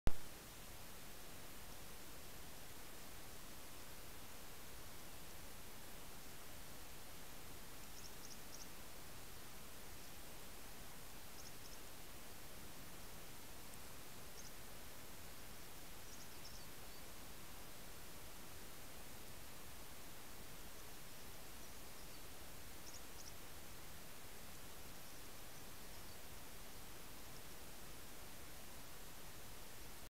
Eurasian red squirrel eating 2 . Spring colour April Norway.
Eurasian red squirrel eating. Spring colour April Norway.
Ltl Acorn 6310 Wideangel HD Video Trailcam